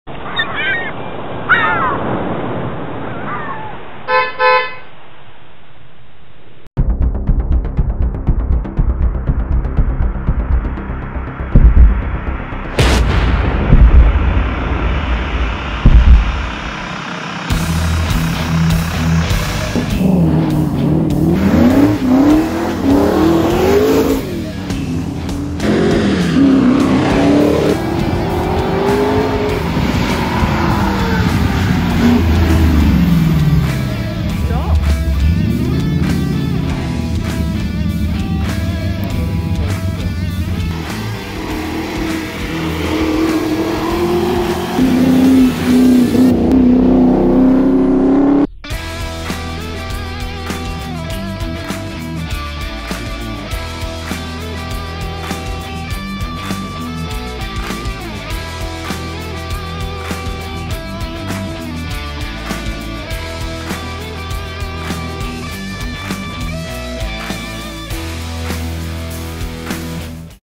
Mustang Coupe Car 57 Ray Law
These are Videos of the Noosa Hill Climb Summer 2020